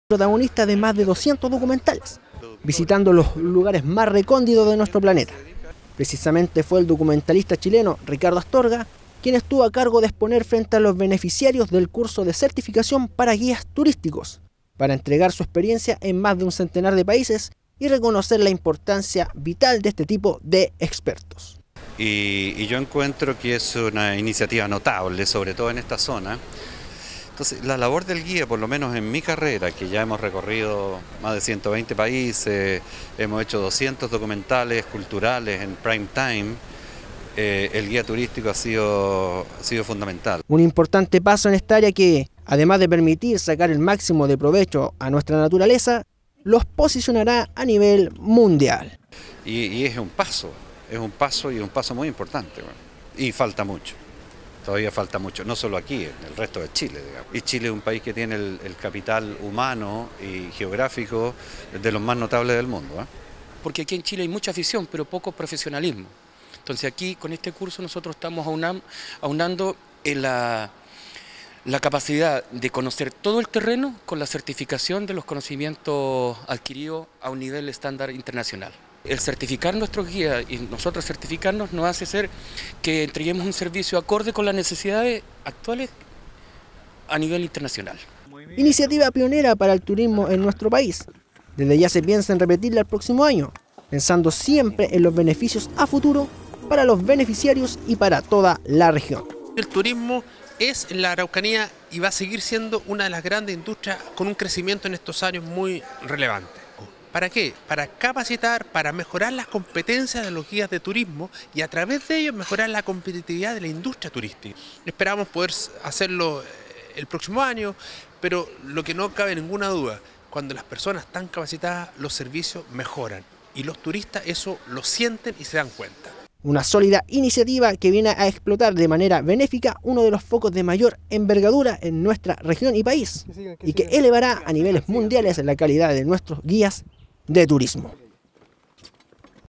GUÍAS DE TURISMO
El turismo es una actividad creciente en el país y requiere profesionalizarse. Con ese objetivo, en la Araucanía se inauguró el curso de guías de turismo, que contó con la asistencia de más de 100 beneficiarios. El informe es de Universidad Autónoma de Chile Televisión.